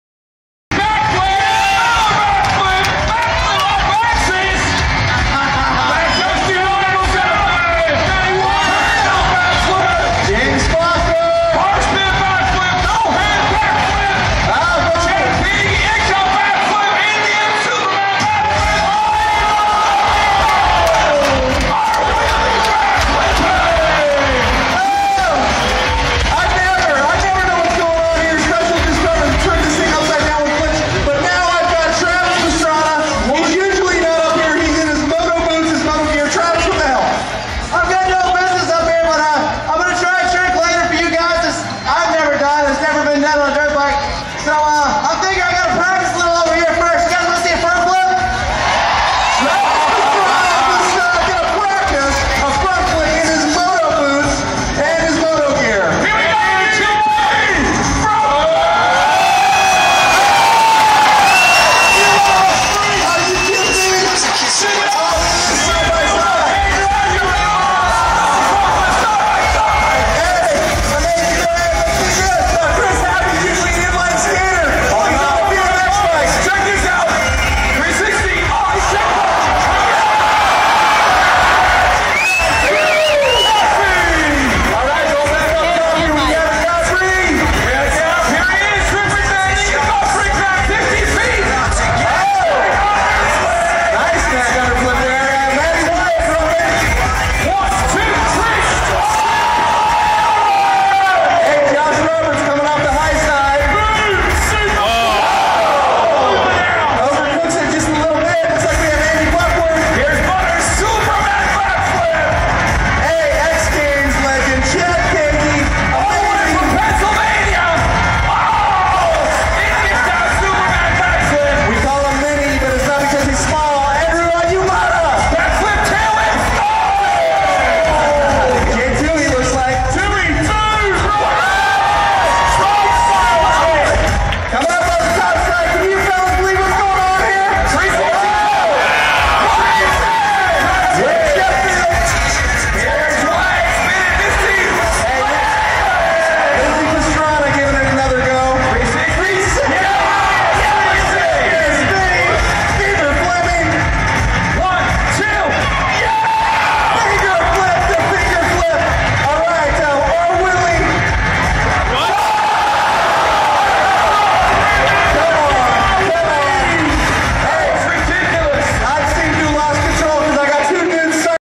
Nitro Circus Live Tour @ MSG in NYC - Vid #1
Various bikes, skaters doing backflip variations. Then Travis Pastrana comes out and does a "practice" front flip on a BMX bike in his FMX gear. Then a couple more flip variations are done.